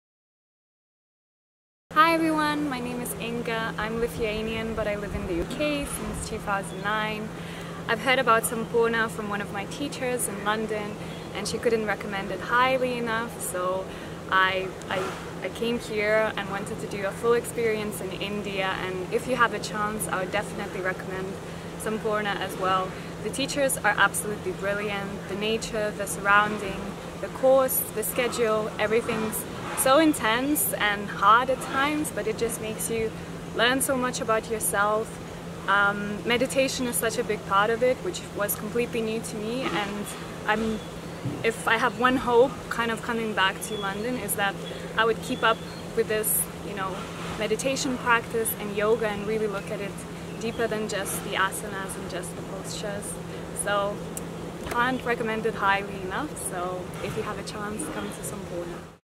Sampoorna Yoga - Yoga Teacher Training - Testimonial - INGA
INGA, an ex-sampoorna yoga teacher training student speaking about her experience with Sampoorna Yoga, India.